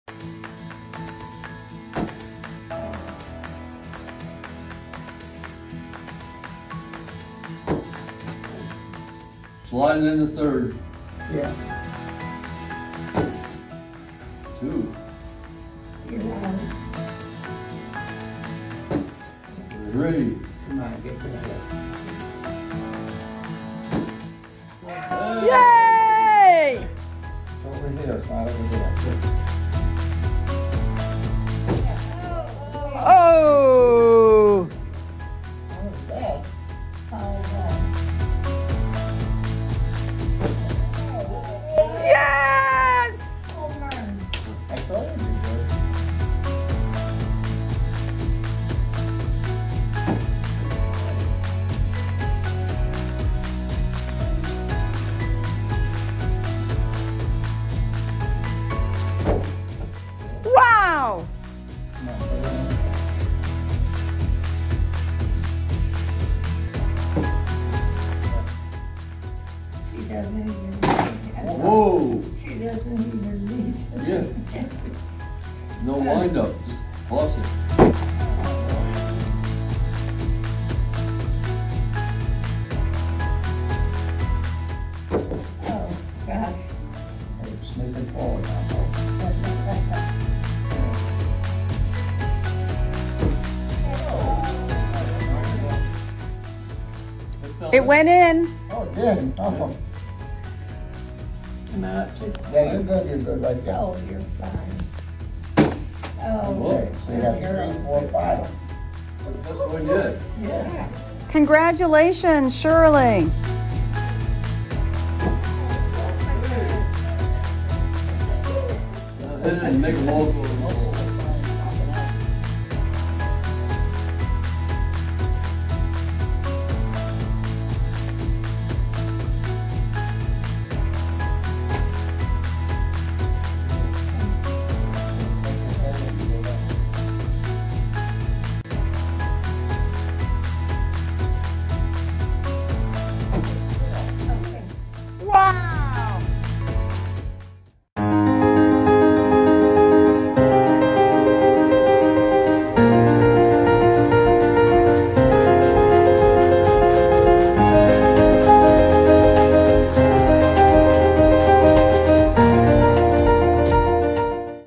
Cornhole With Roxi and our Assisted Living Residents
Join Roxi and our assisted living residents for a fun game of Cornhole! This classic outdoor activity brings everyone together for friendly competition, laughter, and fresh air. Cornhole is a wonderful way for residents to stay active, improve coordination, and enjoy the simple joys of the game.

Chateau Madeleine
📽️🎬WATCH LIVE📽️🎬 ⭐24/7⭐